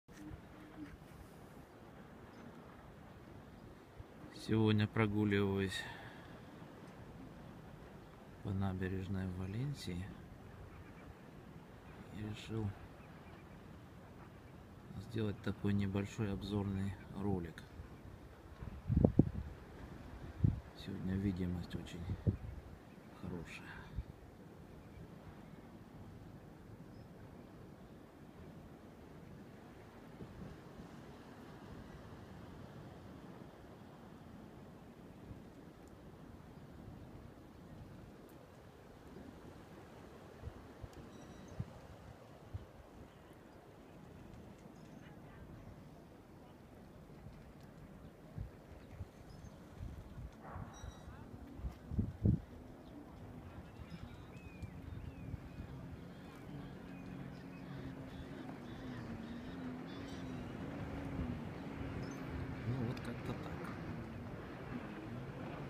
Обзорный вид Валенсии с порта. Валенсия. Испания.
Сегодня гулял в порту города Валенсии, Испания была очень хорошая погода чистый воздух и хорошо дышалось морским воздухом у меня было хорошее настроение. Решил снять небольшой ролик)))))))))

НОВОСТИ О ВАЛЕНСИИ МОЖНО ПОСМОТРЕТЬ НА НОВОСТНОМ САЙТЕ О ВАЛЕНСИИ НА РУССКОМ ЯЗЫКЕ ВОТ ПО ЭТОЙ ССЫЛКЕ